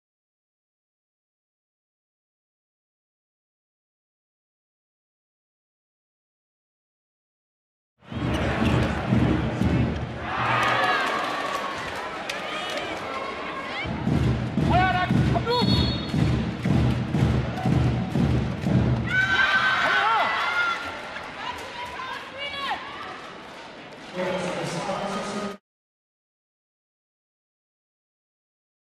Filippa Idehn og resten af holdkammeraterne fra...
Filippa Idehn og resten af holdkammeraterne fra Silkeborg-Voel KFUM lukkede kun 15 mål ind, da holdet forleden sejrede ude over TTH Holstebro ved at score dobbelt så mange mål som hjemmeholdet. 15-30 endte opgøret, i hvilken Idehn noterede sig for en redningsprocent i frit spil på hele 58. Imponerende!